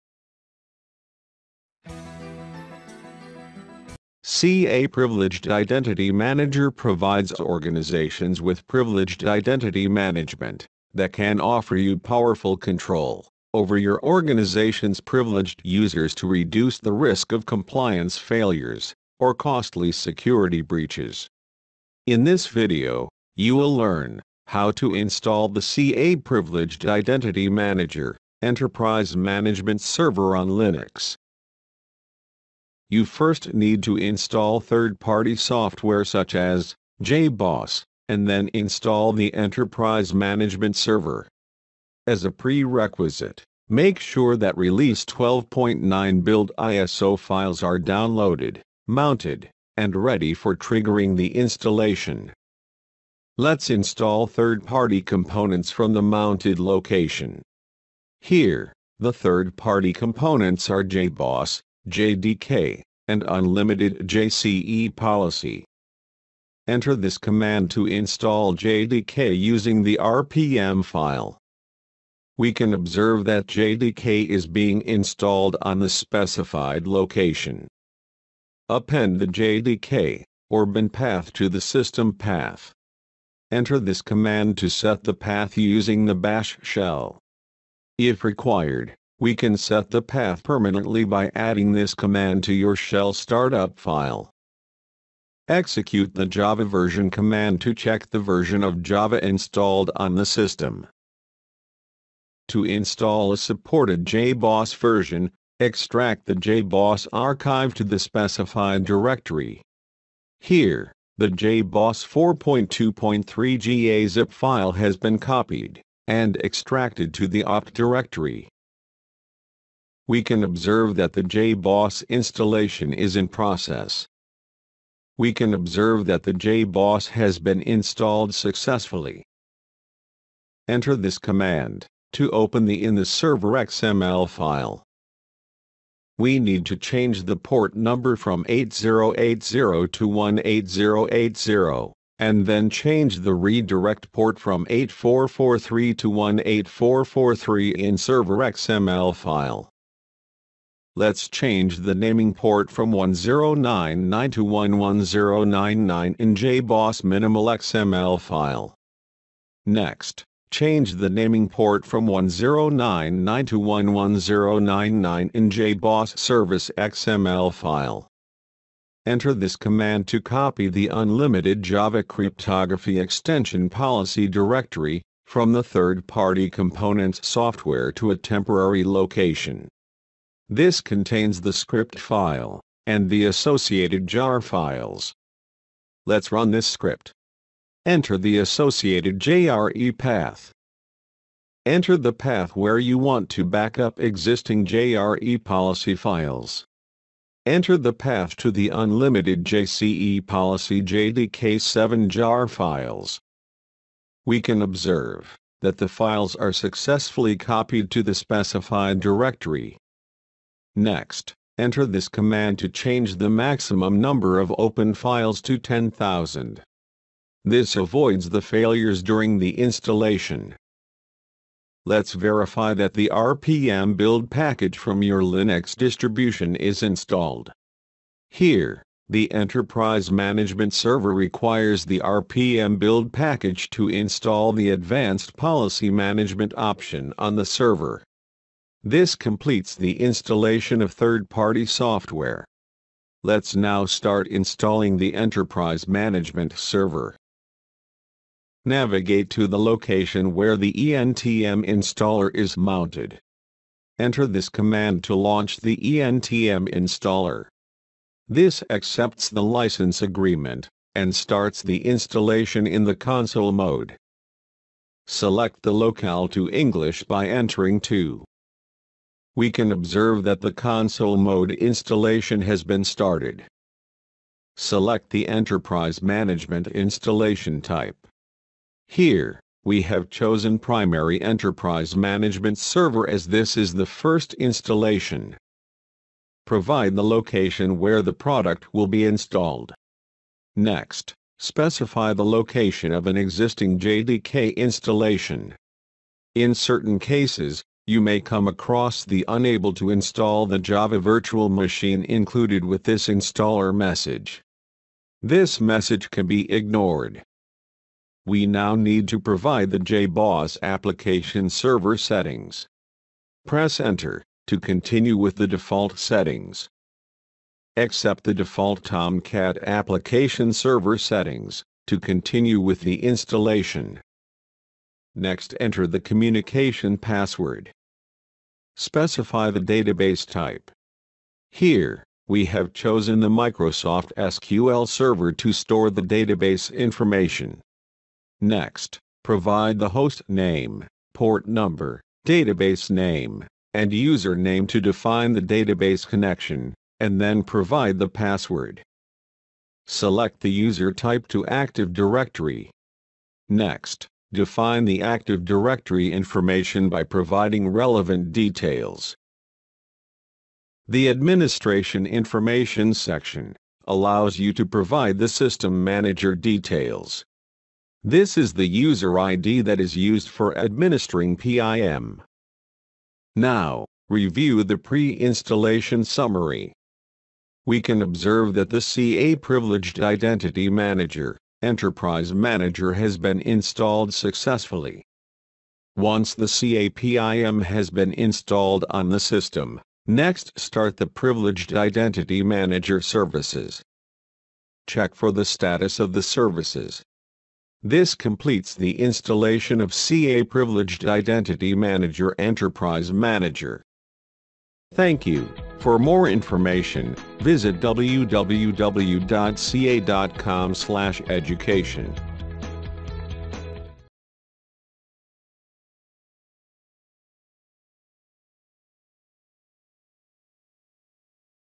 CA PIM 12.9: Install CA Privileged Identity Manager on Linux
CA Privileged Identity Manager provides organizations with privileged identity management (PIM) that can offer you powerful control over your organization’s privileged users to reduce the risk of compliance failures or costly security breaches.
In this video, you will learn how to install the CA PIM Enterprise Management Server on Linux.